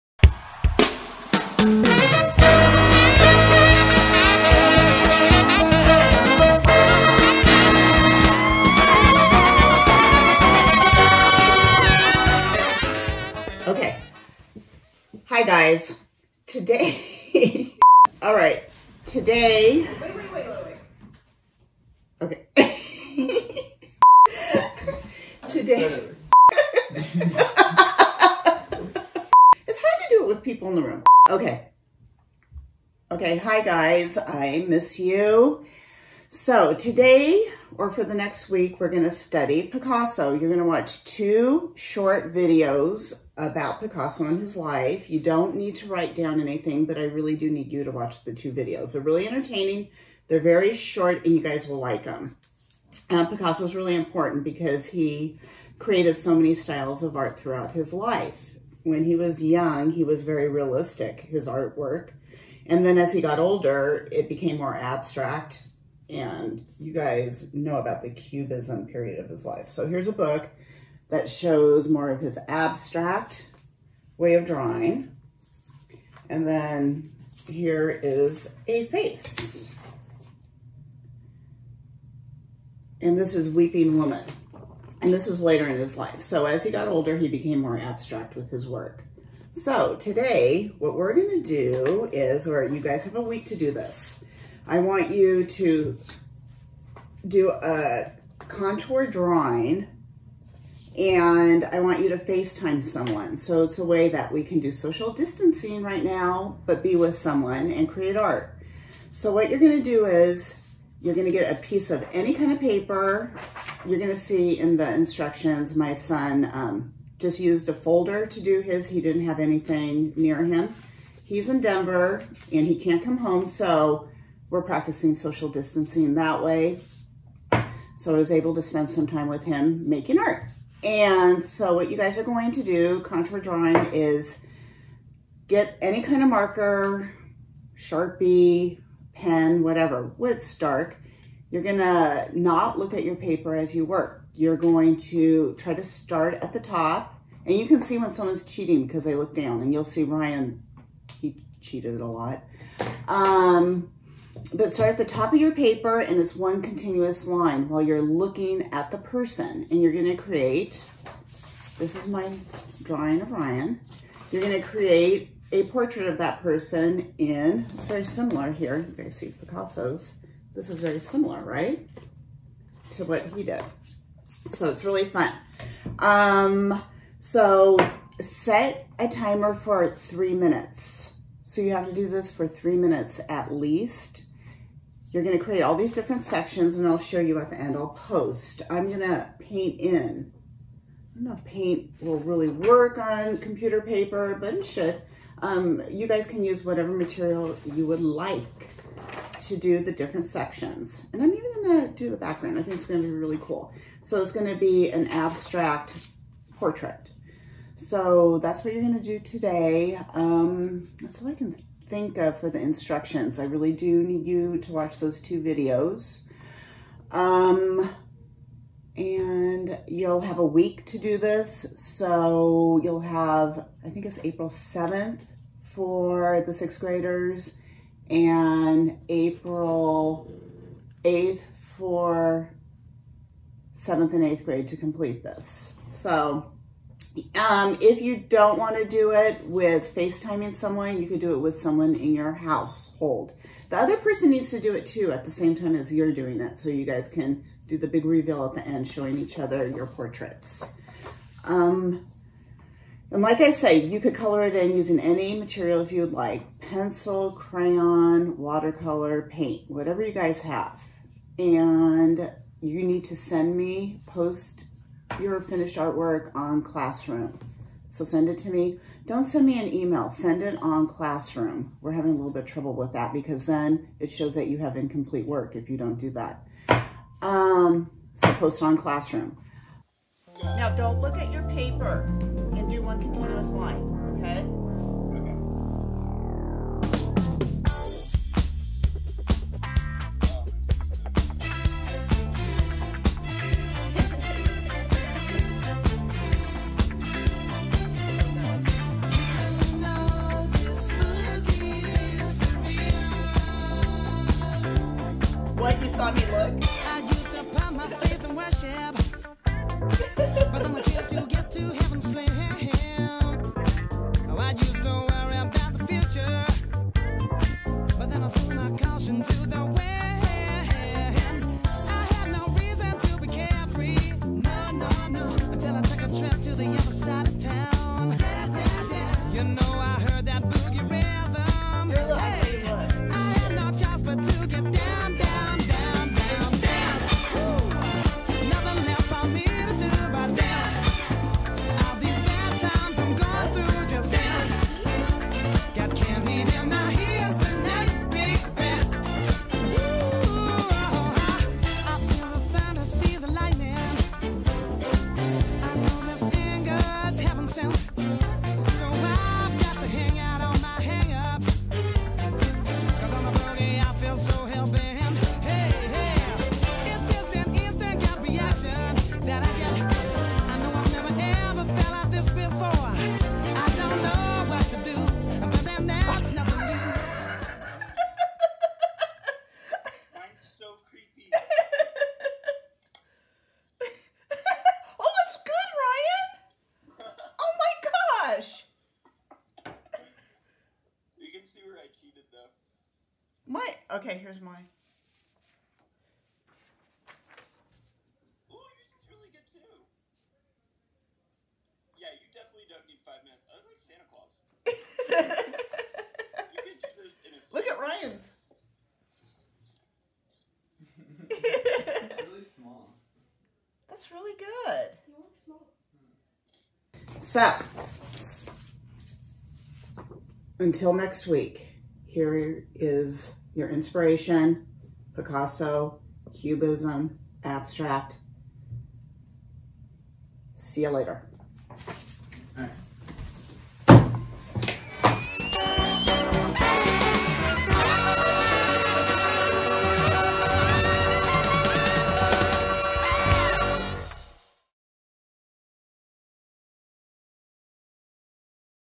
Mrs. Renn Art Class #2
The drawing exercise for this week will be to either draw someone in your house or, even more fun, Facetime one of your friends and both draw at the same time! The way to contour draw is to look at the person you are drawing and not look at your paper as you draw.

You will need a piece of paper, computer paper is fine, a black marker, sharpie or whatever you have and any coloring tools, crayons, markers, water color.

YOU CANNOT LIFT YOUR DRAWING TOOL ONCE YOU START, that's why it's called a contour drawing! Your line should be one continuous line.

Set a timer for at least 3 minutes.

Color in each space you have created, it's best to color each space a different color or use a color palette.

Once you are finished, you will have a Picasso inspired cubist portrait!